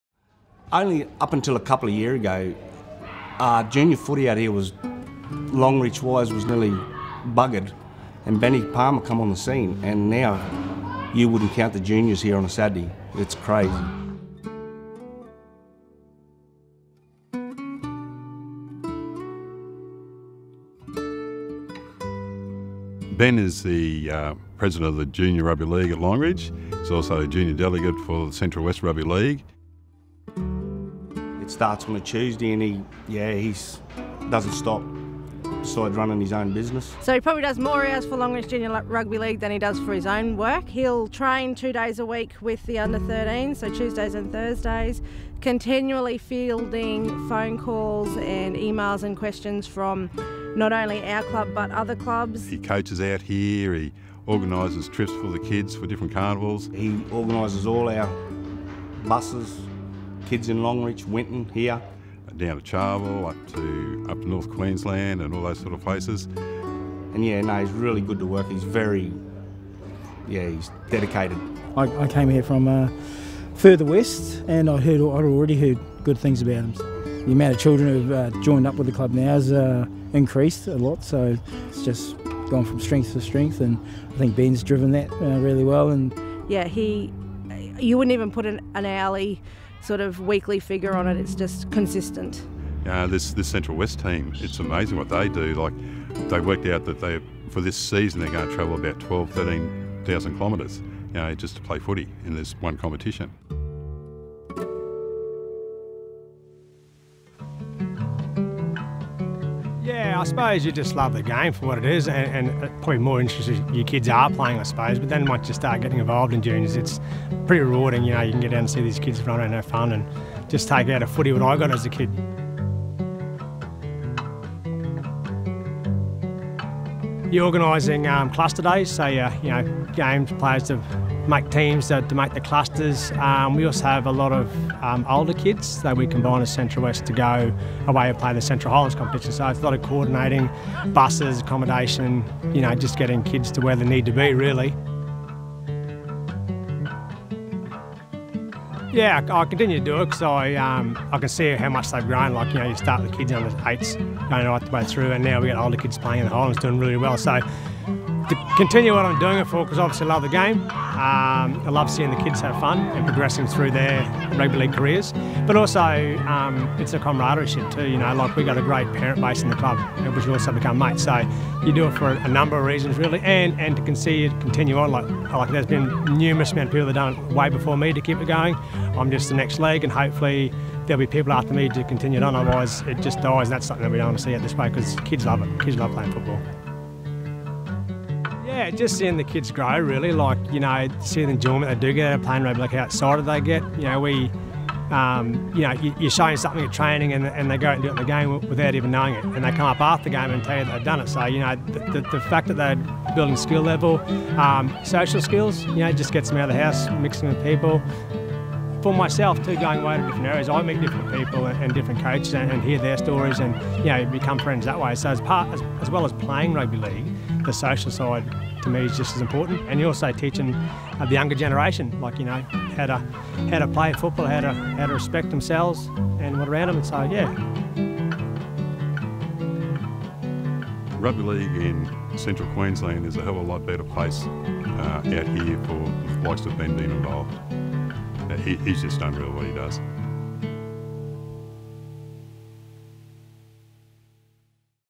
Ben Palmer 'just doesn’t stop’
Longreach Junior Rugby League president Ben Palmer is a passionate advocate for young players enjoying the game and helps keep the rugby league flame brightly burning in the Outback.